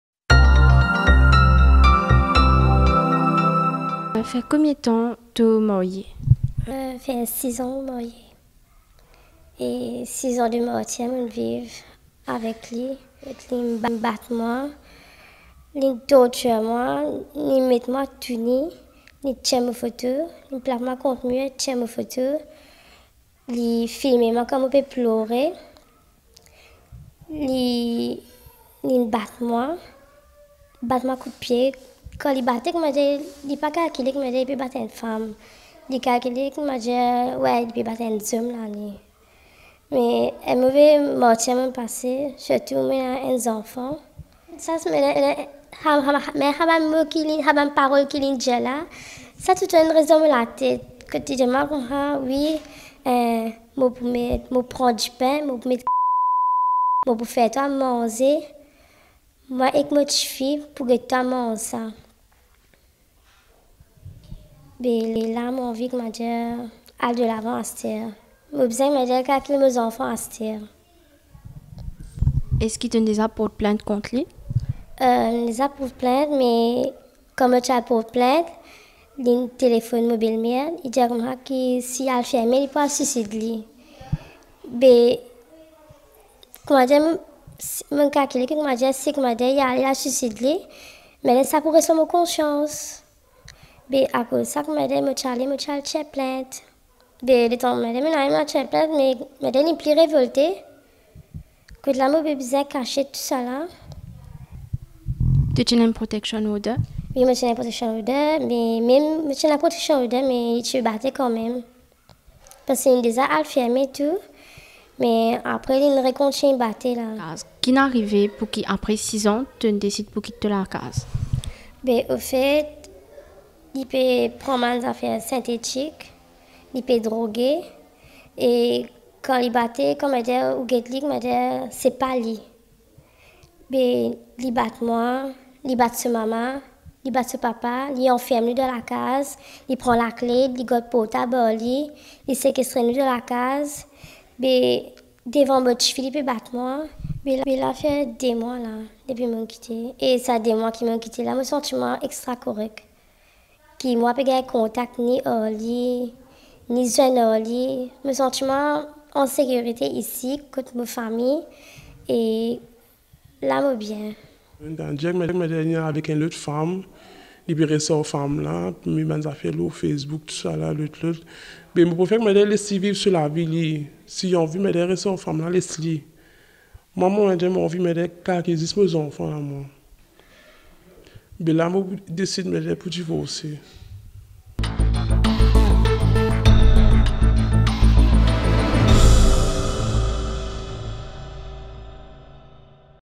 Humiliée et maltraitée, elle s’enfuit de chez elle après six ans de martyre
Belle, jeune et avec tout pour plaire… Avant qu’elle ne nous raconte son histoire, nous n’aurions pas cru les horreurs qu’elle aurait pu vivre. Gloria (prénom fictif), s’est enfuie de chez elle avec sa fille et sa belle-mère il y a deux mois car n’en pouvant plus des coups et des humiliations. Son mariage a duré six ans.